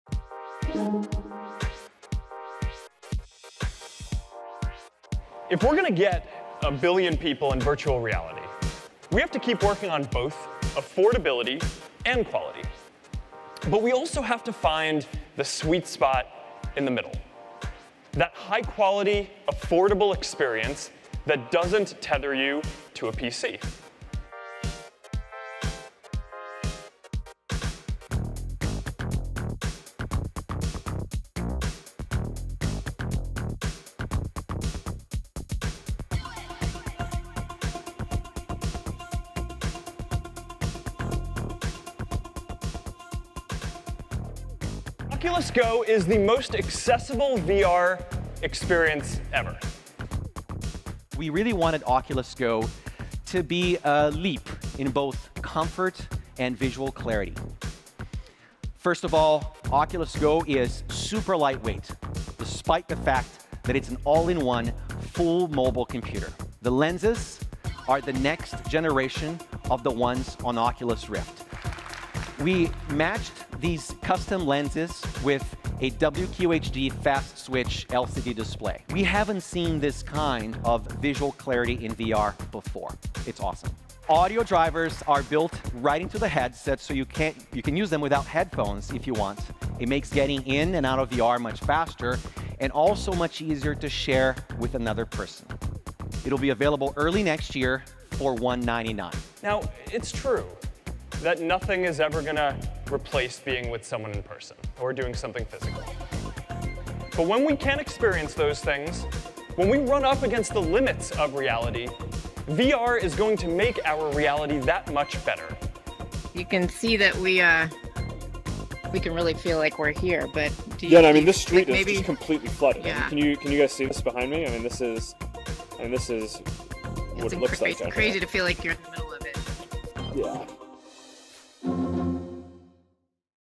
Facebook unveils standalone VR headset, Oculus Go
On Wednesday, Facebook announced the Oculus Go, a virtual reality headset that works without tethering to a phone or computer.